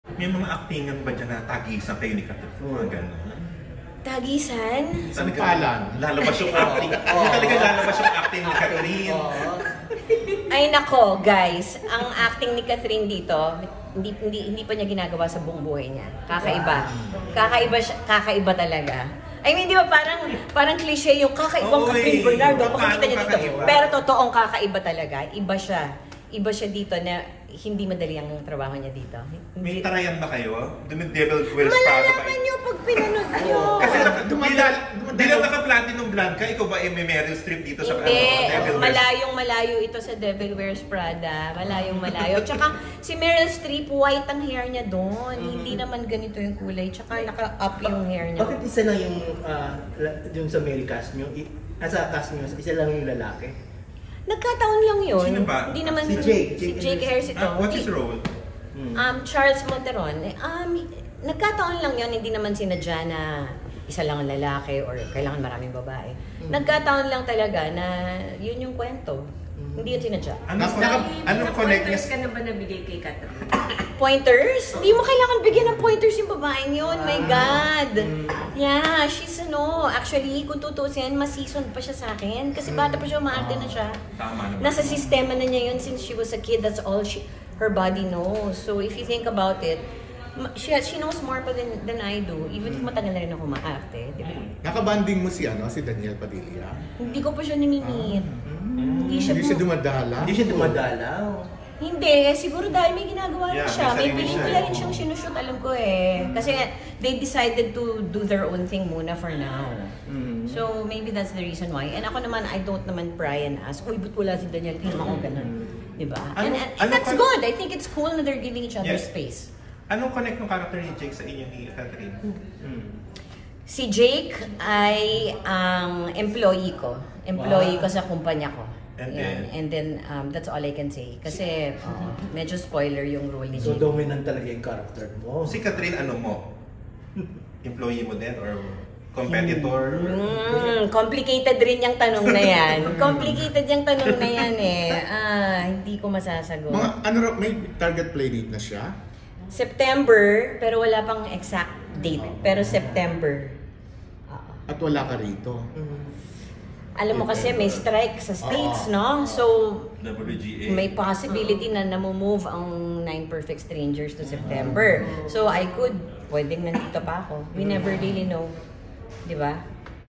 MS. DOLLY: “KAKAIBANG KATHRYN! Ang AKTING Niya Rito, Hindi Pa Niya NAGAGAWA sa Buong BUHAY Niya!”
Intimate interview with Ms. Dolly de Leon at the Cinemalaya 2023 presscon held at the PICC. July 5, 2023.

Ms. Dolly has two entries in this year's Cinemalaya filmfest--'Iti Mapukpukaw' with Carlo Aquino and 'Ang Duyan ng Magiting' with Paolo O'Hara.

Cinemalaya XIX: ilumiNasyon will run from August 4 to 13 at the PICC and selected Ayala Cinemas.